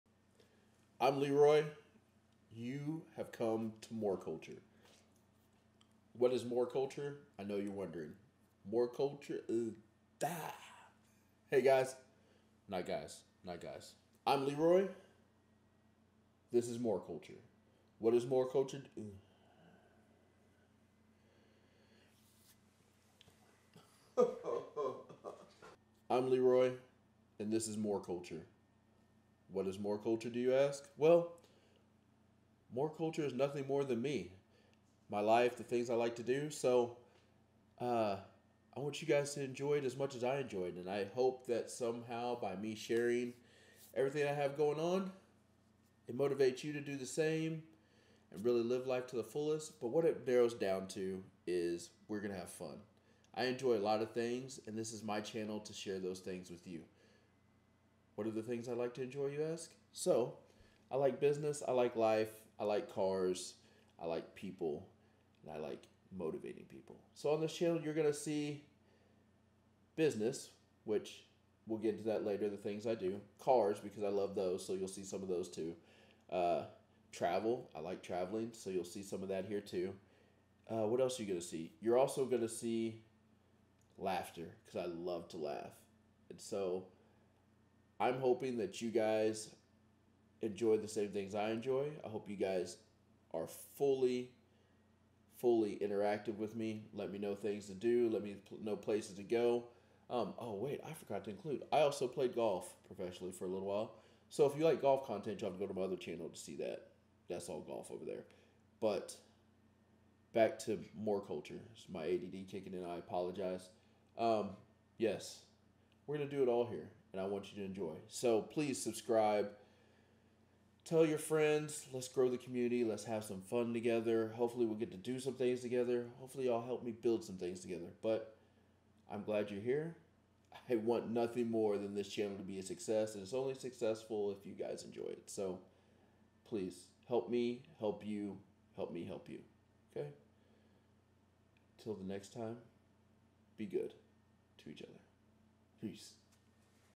MOORE culture | I AM LEROY! | THIS IS MOOR CULTURE
For those that have noticed. I have rebranded my YouTube channel. It was previously known as Everything Fabrication. But after 10 years in the YouTube game, it is time to be authentically myself. That person is multi dimensional. With a lot of irons in the fire. My goals are big, and  I want to enjoy the journey, with you guys. From business to travel, and a love for cars. It is all going to be here. You definitely will be able to find something you enjoy.